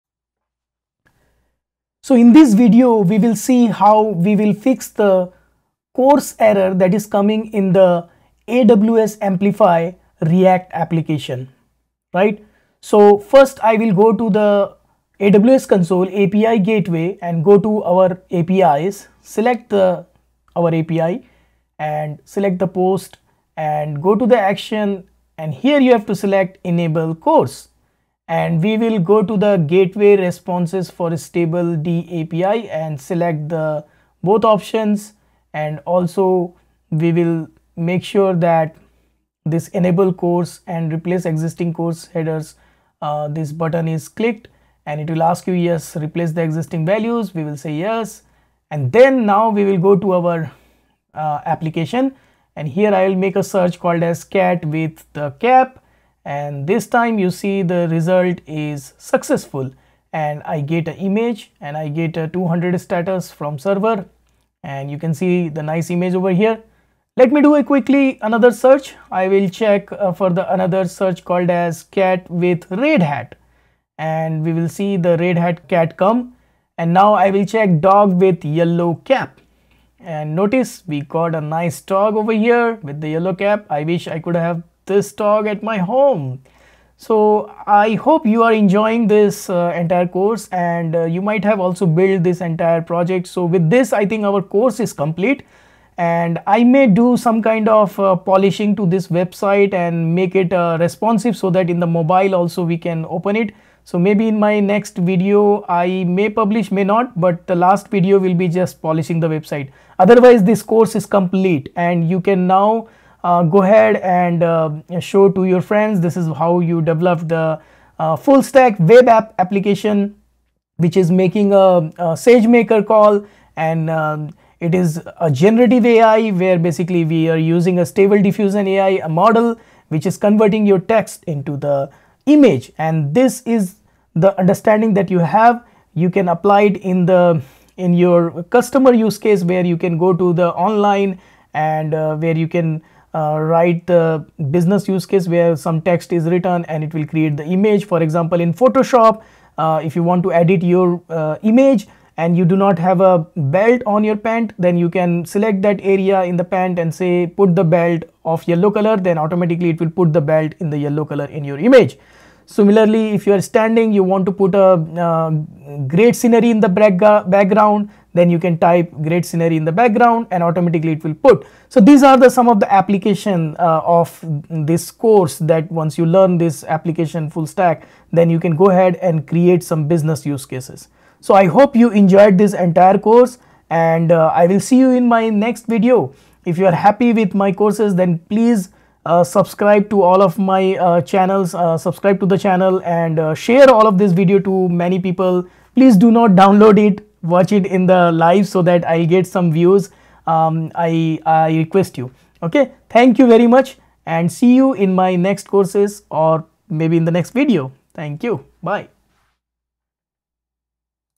Fixing CORS Error and Running Gen AI Successfully in AWS | Full Stack Generative AI | Video 23
Join me for the final video of this series as I address the CORS error and successfully run the Generative AI application in AWS. Witness the completion of the full stack application, utilizing AWS services to create a powerful Generative AI web application powered by the stable diffusion model.

Throughout this course, we have explored the intricacies of building a full stack application in AWS, leveraging the capabilities of diffusion models to generate images from text inputs. Now, in the last video, I will focus on resolving the CORS error and ensuring a seamless execution of the Generative AI application.

Diffusion models, such as dalle or imagen, have gained significant attention in recent times as powerful generative models. This video serves as a culmination of our journey, showcasing the successful implementation and execution of a cutting-edge Generative AI solution.

I hope that you have enjoyed this entire course and found it both informative and engaging.

Thank you for joining me on this exciting journey.